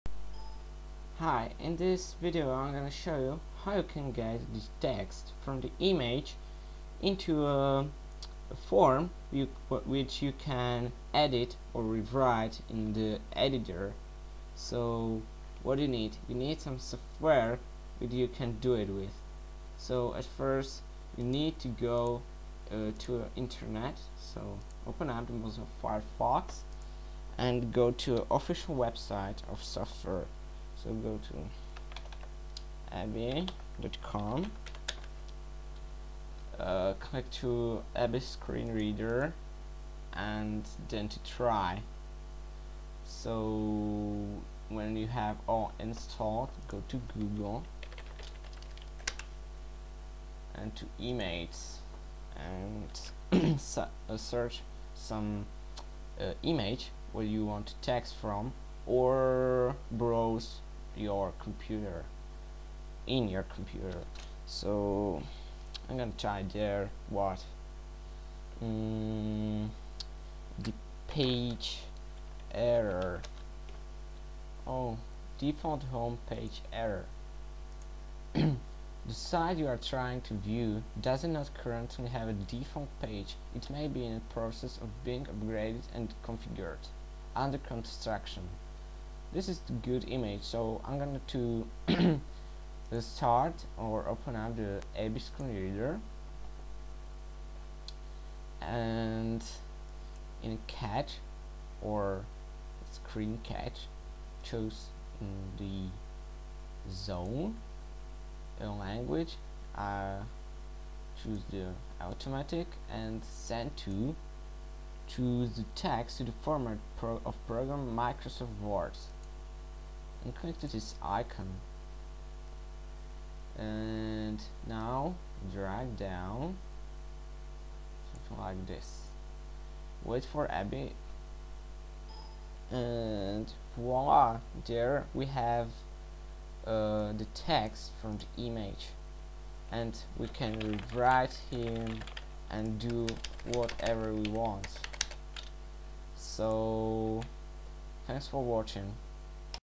ABBYY Screenshot Reader tutorial
Tutorial about how to get text from image into the form which you can edit.